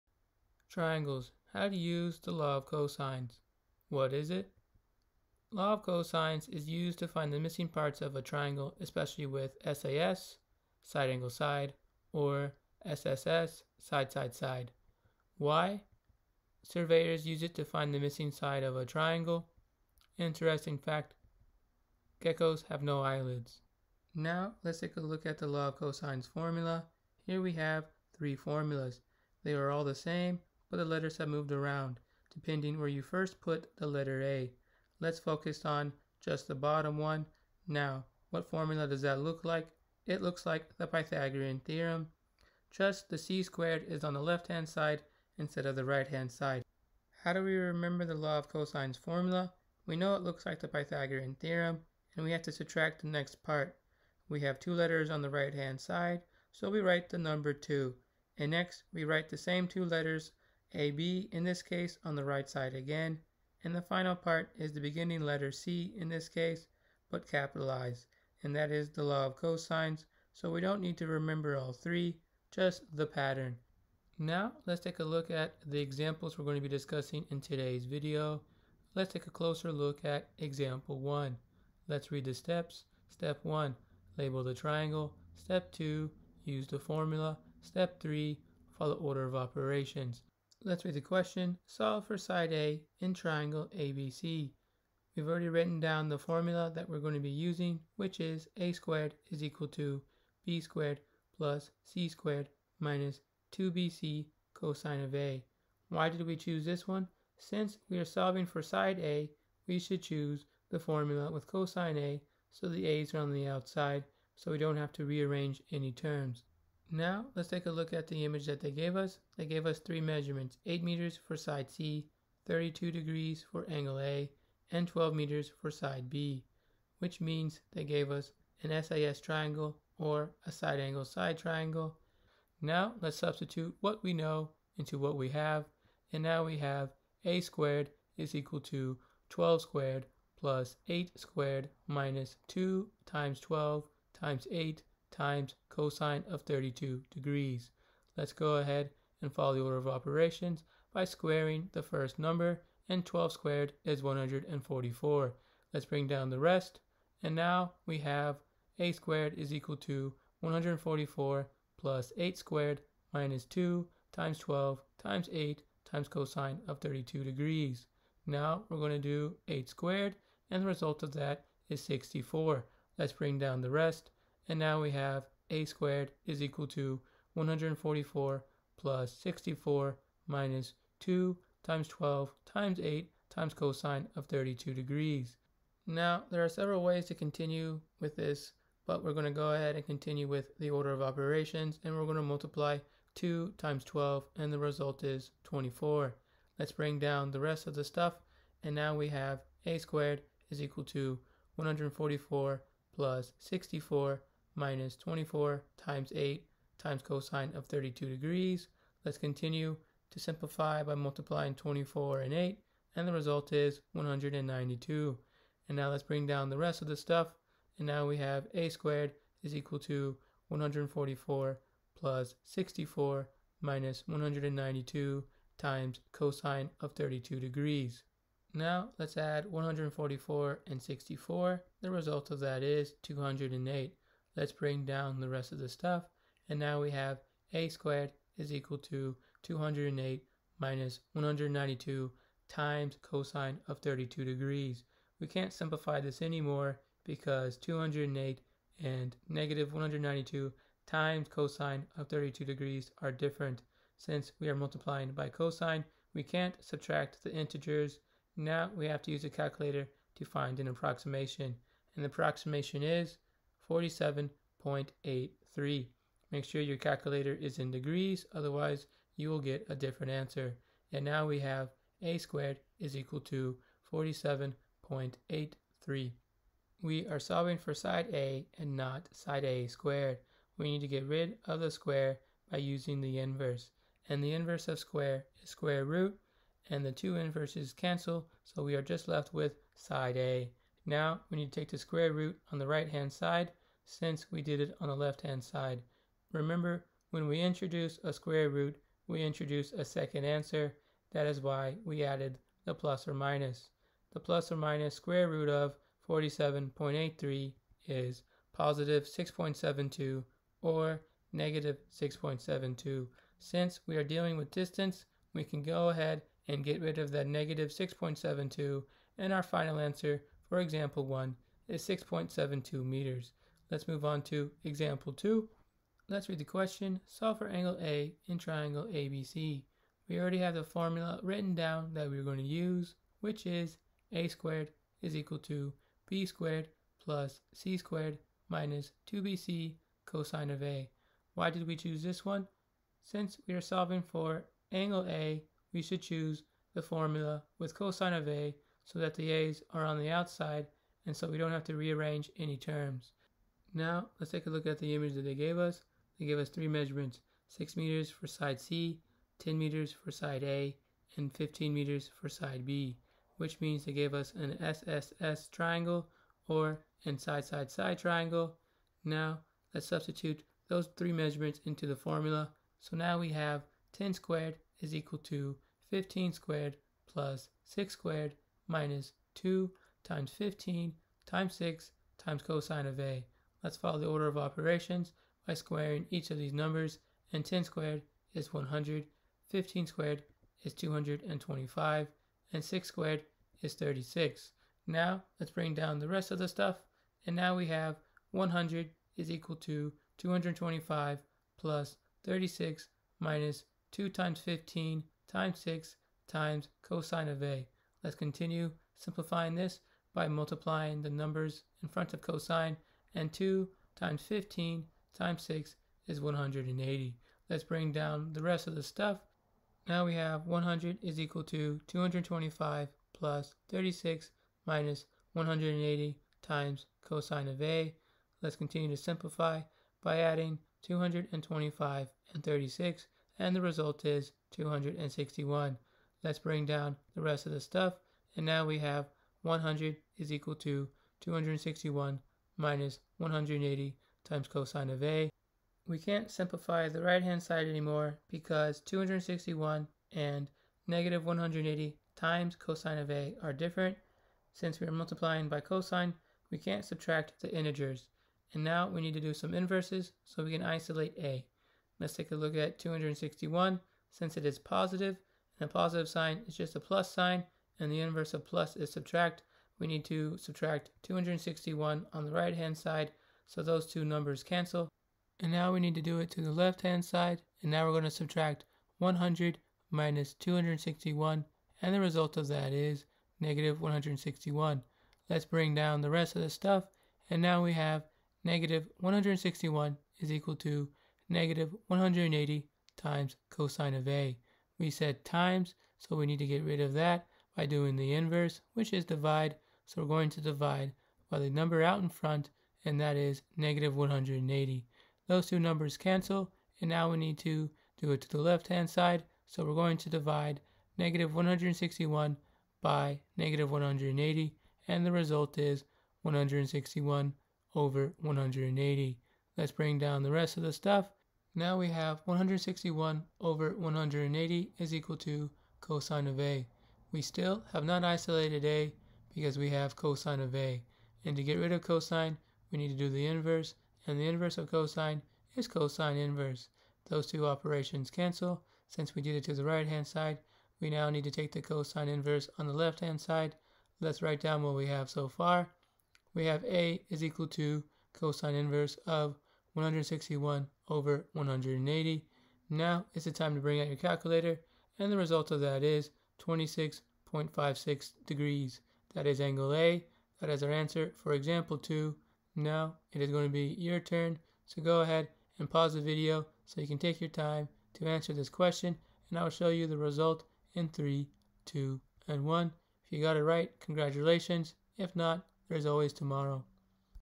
Triangles- How to Use the Law of Cosines - Math is the Way Corner
If you need a refresher on how to use the law of cosines then this video is for you.

Start - 0:00

Formulas - 0:24
Example 1 - 1:23
Example 2 - 5:25
Your Turn - 9:24